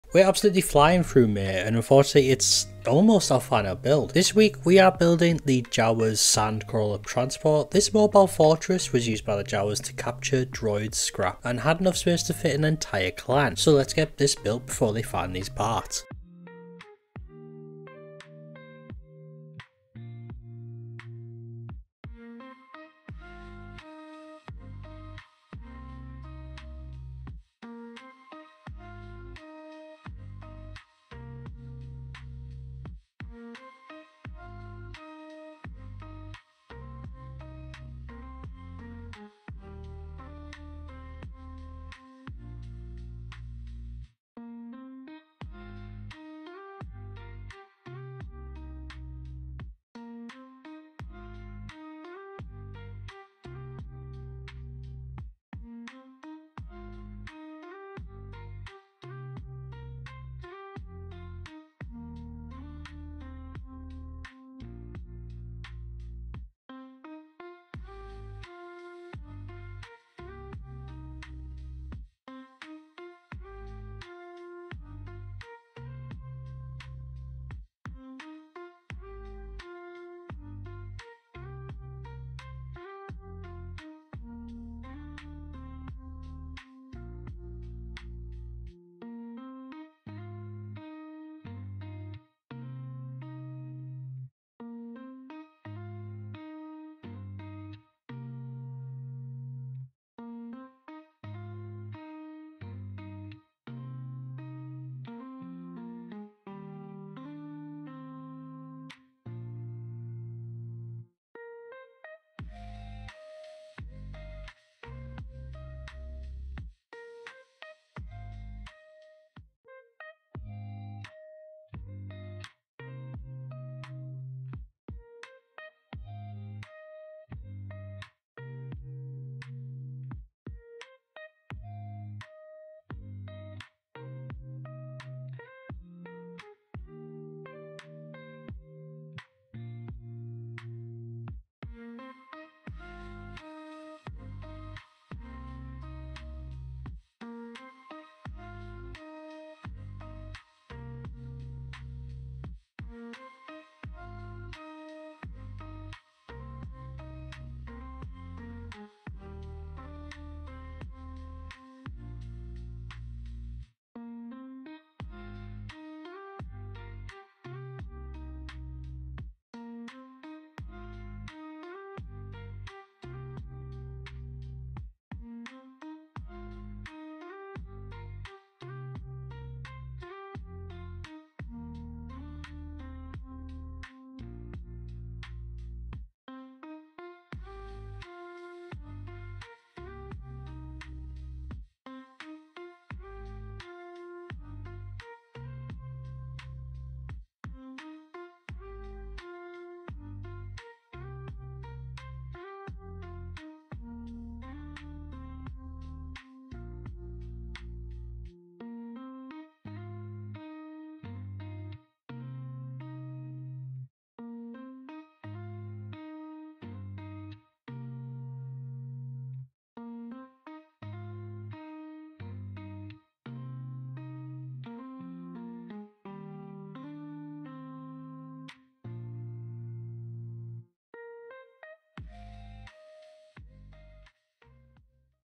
Jawa Sandcrawler - Simple & Quick LEGO Build Tutorial
We're more than halfway through May and nearing the end of the Star Wars builds! This week, we are building the Jawa's sandcrawler. This fortress on wheels allowed the Jawa clan to store droids and scrap. What do you think of it?

Latest Lego Deals